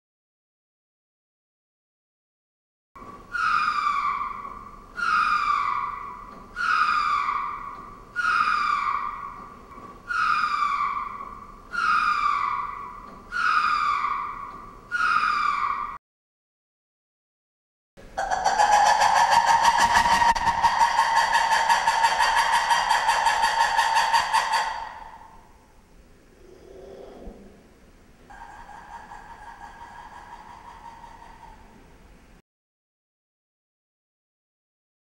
Lemur Sounds
Lemurs make lots of different sounds. Can you guess the type of lemur based on the sound that it makes? Listen to these two lemurs and test your lemur knowledge! To listen to Lemur Sounds PART 2, please The new collection includes vocalizations of sifakas, aye-ayes, ring-tailed lemurs, mouse lemurs, bamboo lemurs, and more!